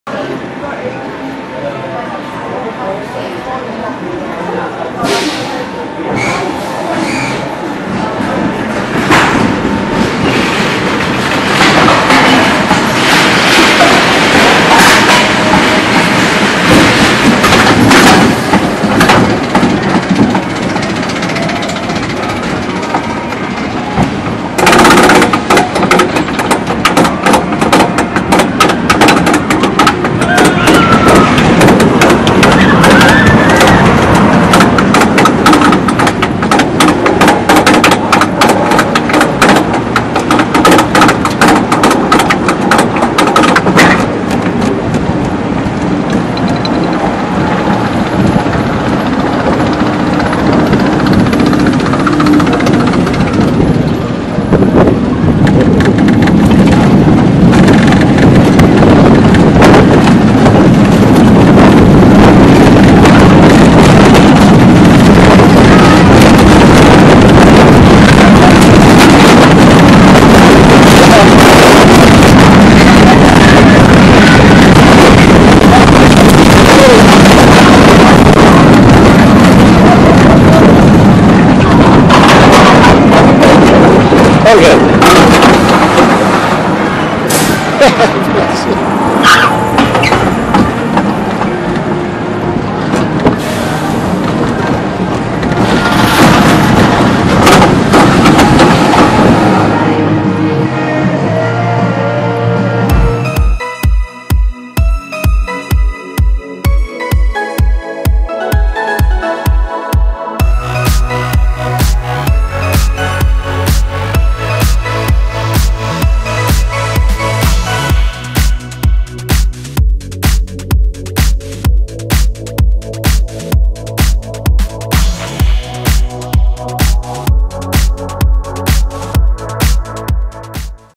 Blackpool Pleasure Beach Avalanche Roller Coaster On Ride Front Seat POV
Avalanche is the first steel bobsled roller coaster constructed in the United Kingdom. It was built by Mack of Germany in 1988 at Pleasure Beach Blackpool, an amusement park in Blackpool, England. Wikipedia
Address: 525 Ocean Boulevard, Blackpool FY4 1EZ, UK
Height: 10′
Height restriction: 3′ 8″
Max speed: 50 mph

Filmed with a 2k h.264 Spywatch